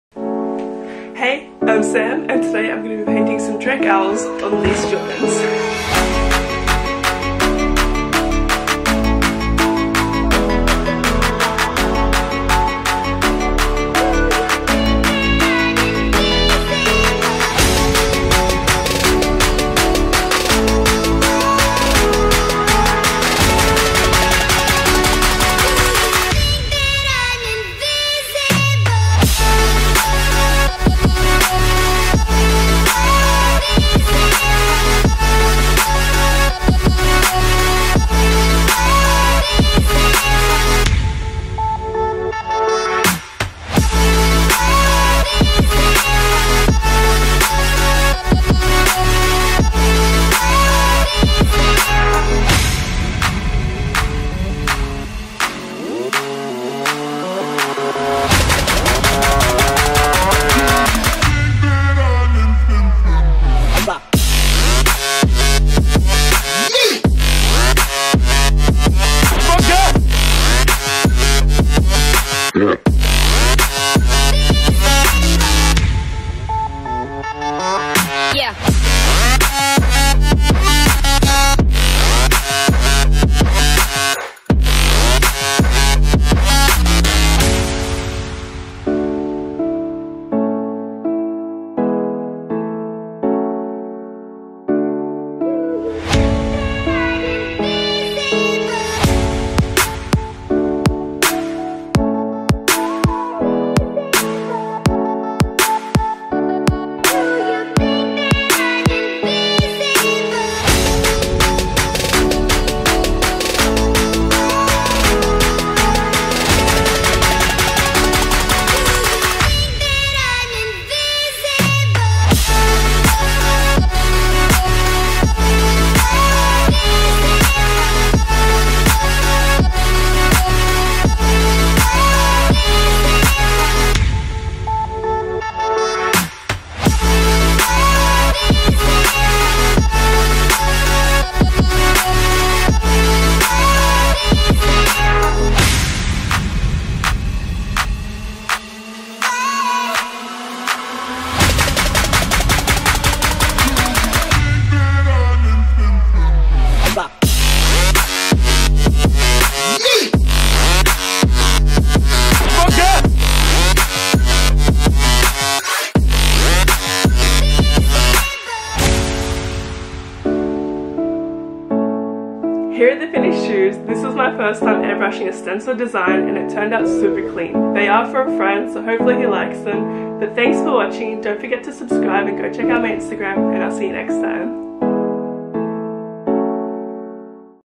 🦉 Custom Drake OVO Owl Air Jordan 1's 🦉
I painted some OVO Owls on these Air Jordan's for a friend :)